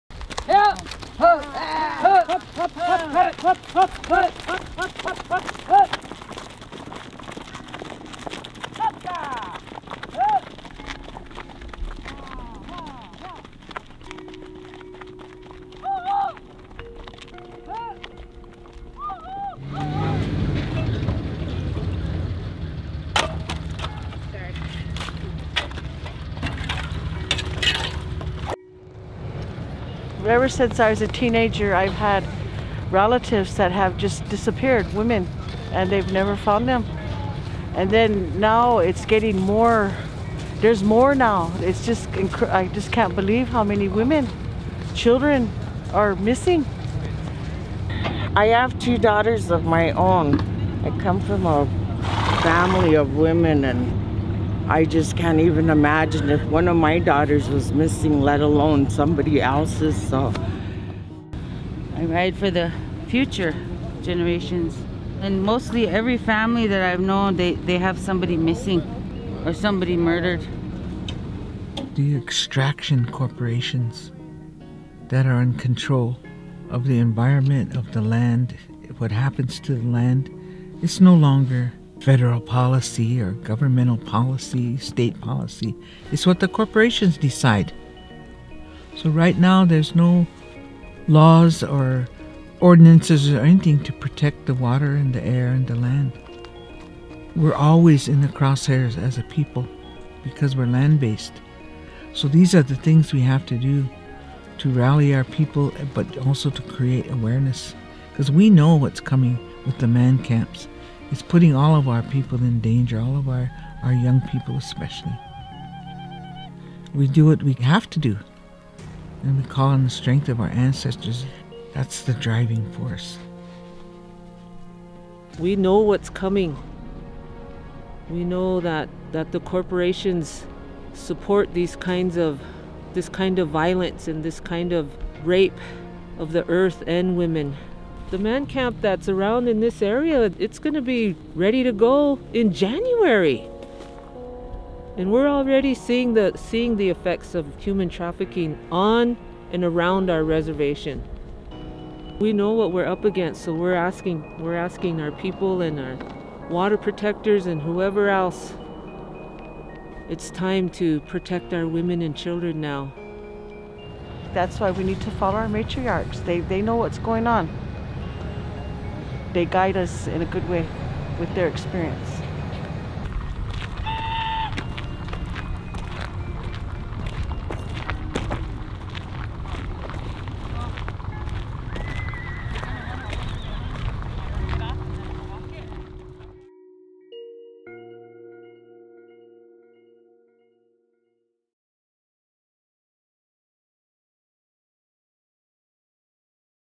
Dawn Decora - W.A.R.N. Ride
In December a group of women from women from the Fours Bands of the Oceti Sakowin - the Minnicoujou, Itazipco, Siha Sapa, and Oohenumpa - made a symbolic horse ride on the western edge/border of the Cheyenne River Reservation where a man camp will be constructed nearby. This is a stand against the violence and human trafficking that will increase with the establishment of this "man camp" near the border of the reservation. The ride was an idea of some of the original members of Women of All Red Nations (W.A.R.N.), a Native women's activist group founded in 1978 to fight for and protect the lives of Native women, families, and the Earth. The goal is to put out the message that it is time to wake up and be ready to protect the sovereign borders of the reservation nation and everyone vulnerable to the impact of the TransCanada KXL Pipeline's temporary workforce housing commonly known as "man camps." While the building of the pipeline is a significant act of violence to the Earth, this violence translates directly to human trafficking. See the for more information and resources about these issues.